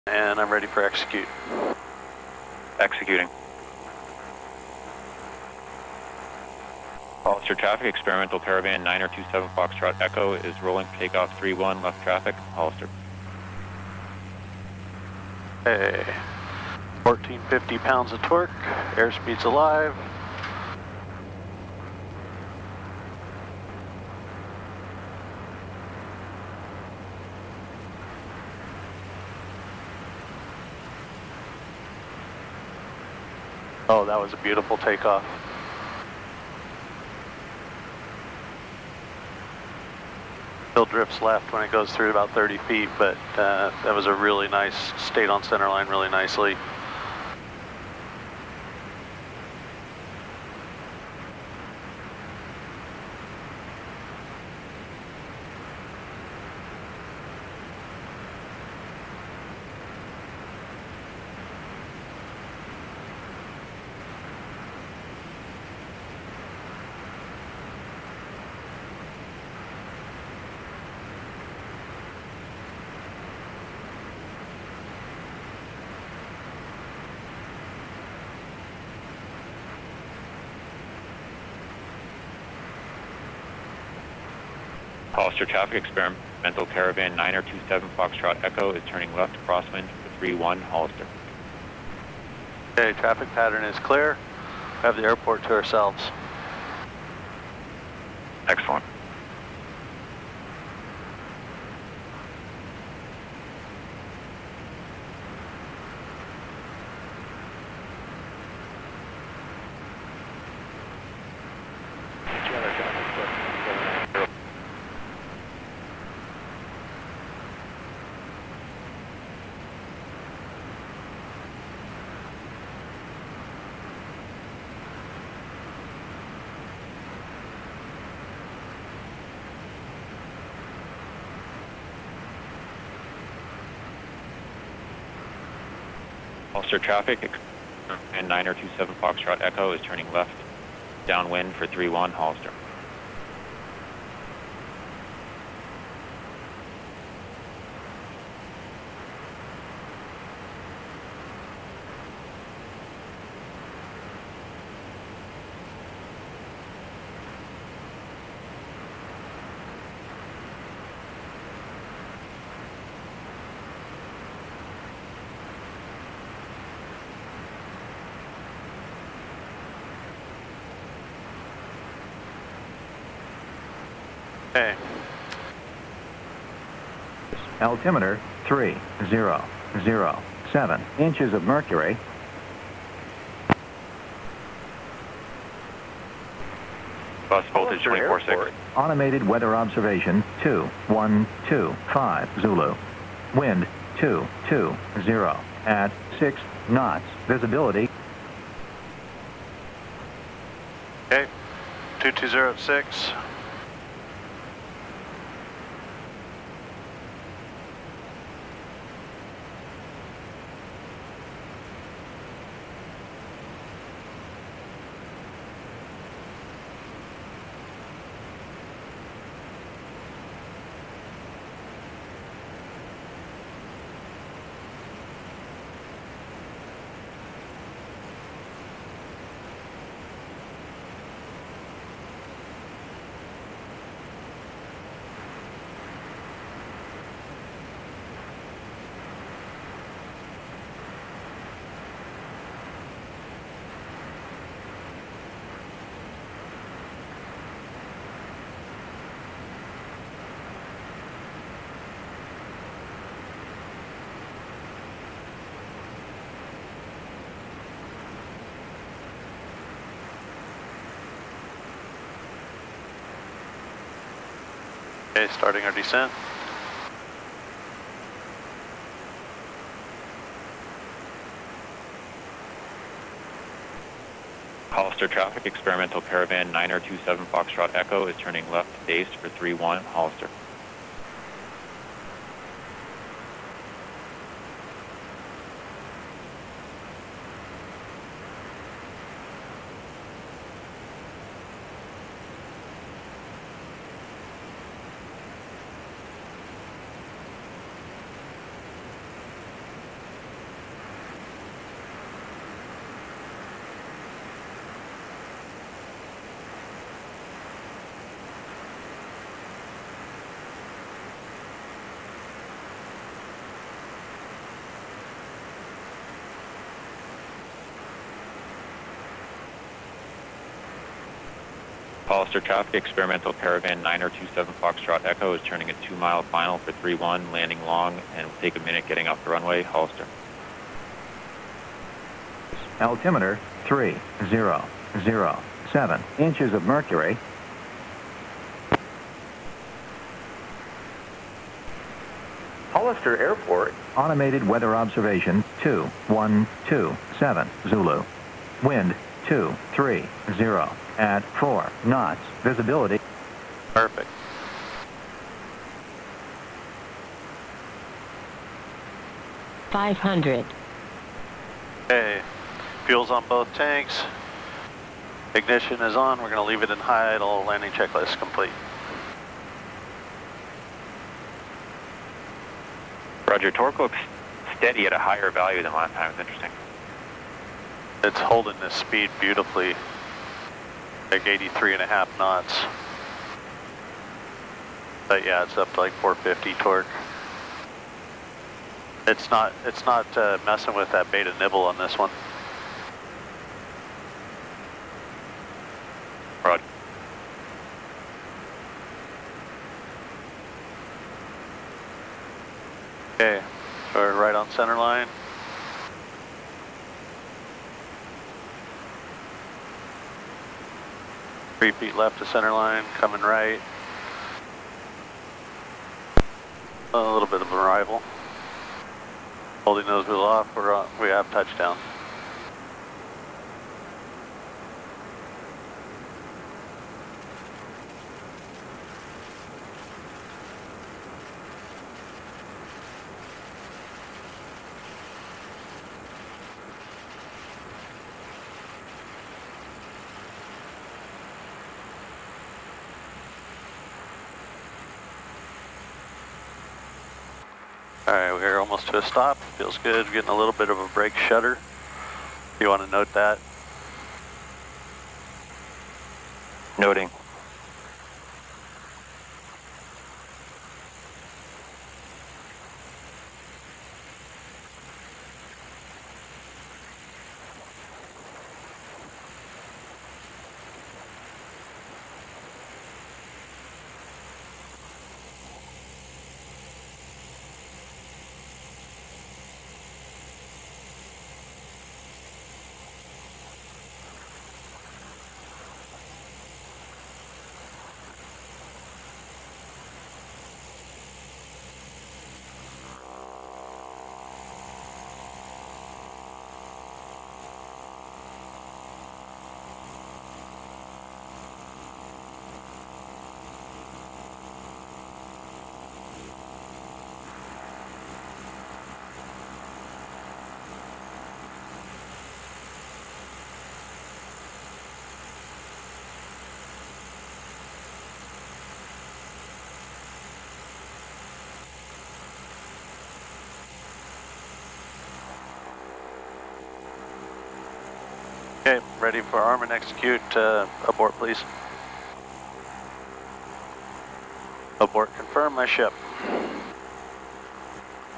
C208 Control Center Demonstration
On February 11, 2021, Reliable Robotics remotely piloted a Cessna 208 from a control center over 50 miles away. The remote pilot managed all communications with air traffic controllers, while the onboard safety pilot monitored the flight.